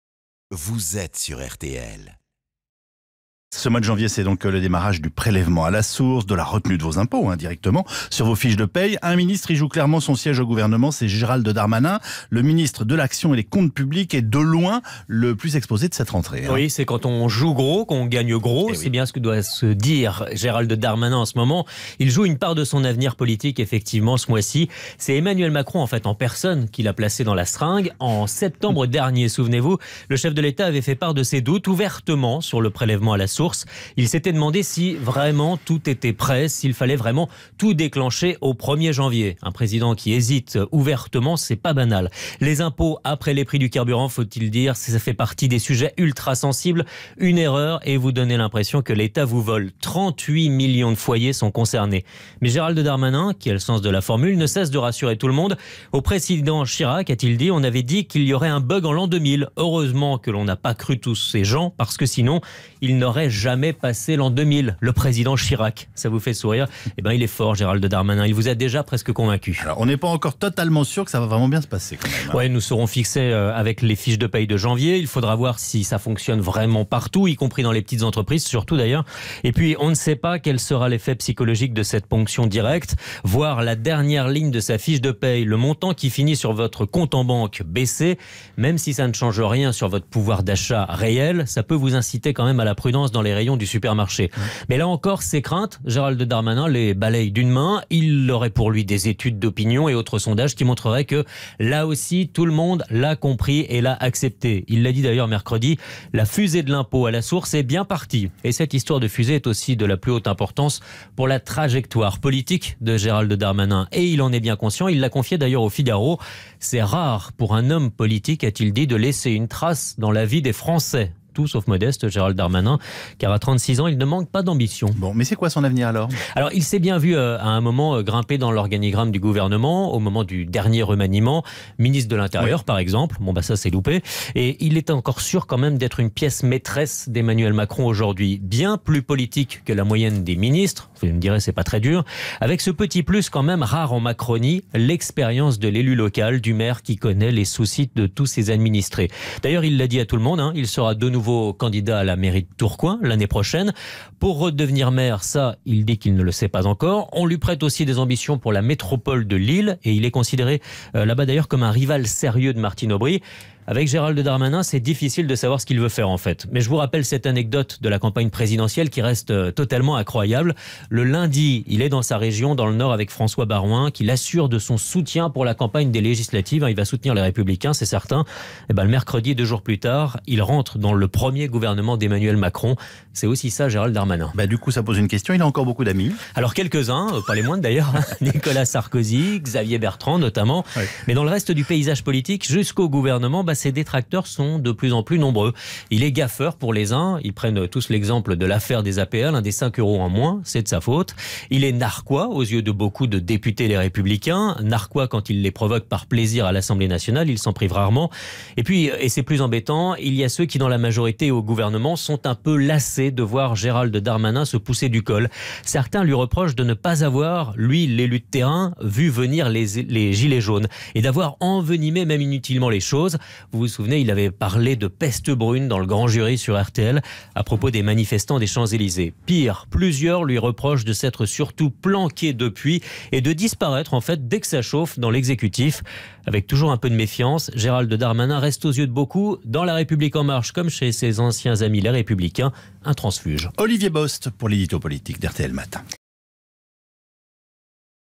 Le prélèvement à la source : une réforme risquée pour Gérald Darmanin
Le ministre joue une part de son avenir politique ce mois-ci. Il faut dire que le sujet des impôts est ultra sensible. Une erreur, et vous donnez l'impression que l'État vole les citoyens.